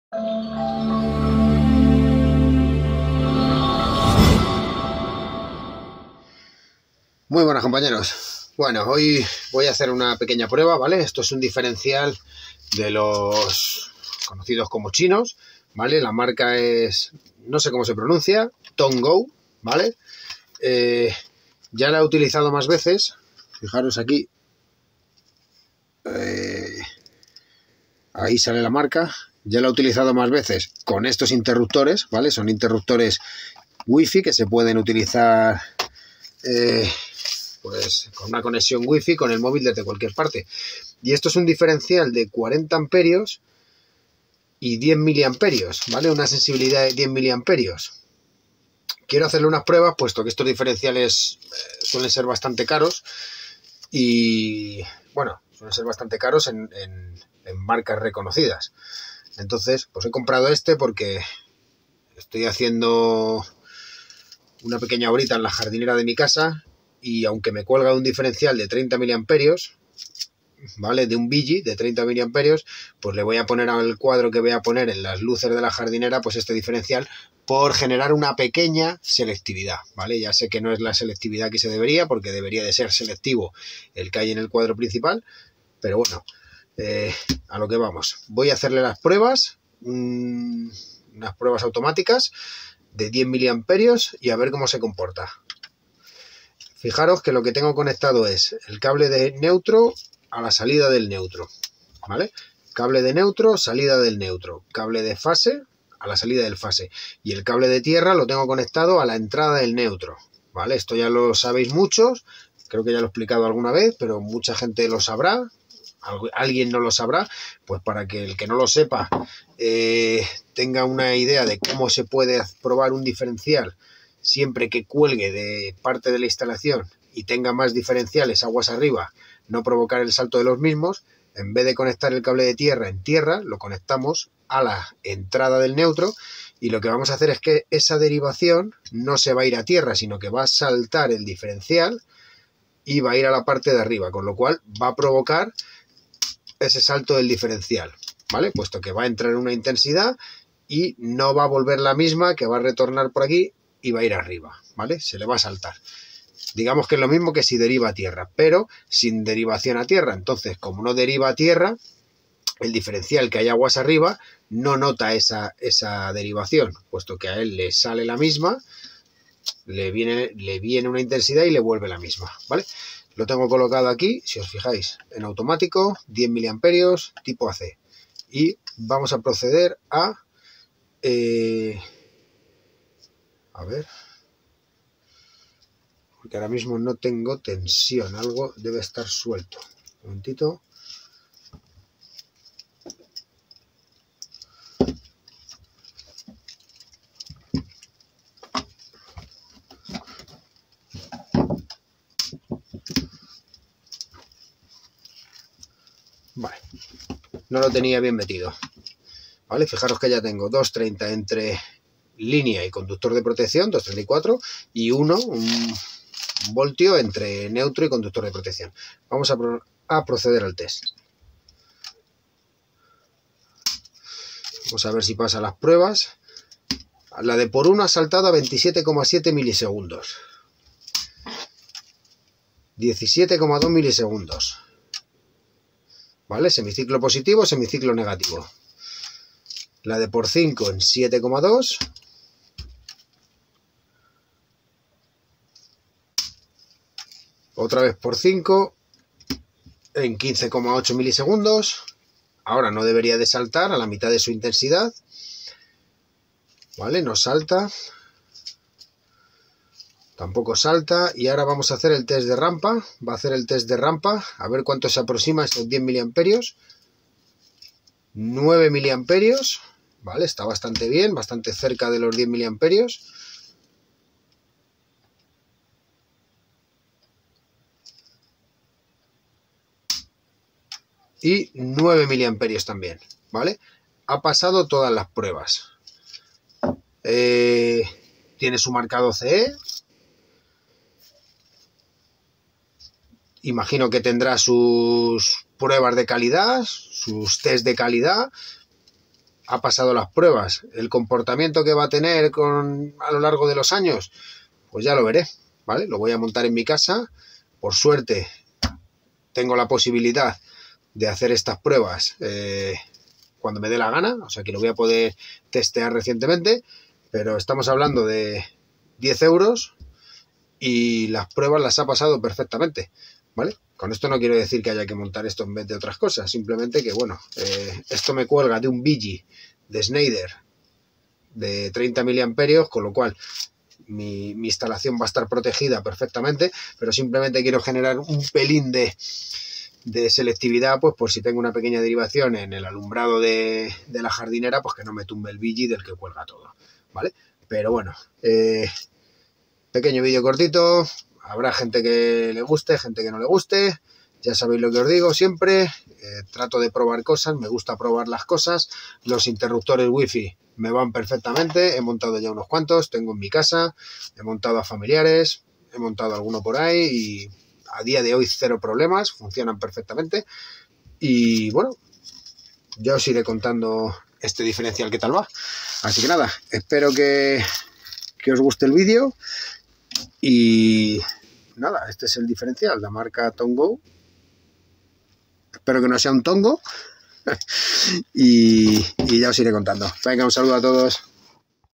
Prueba de diferencial de 10mA TONGOU
Muy buenas compañeros, hoy voy a realizar las pruebas con el equipo multifunción a este diferencial de tipo AC y 10mA para ver cómo se comporta, la marca es una marca china TONGOU  que a priori no parece de mala calidad , las pasara? espero que os guste que comentéis cualquier cosa que pueda mejorar.

Perfil Google
👉Amh-Servicios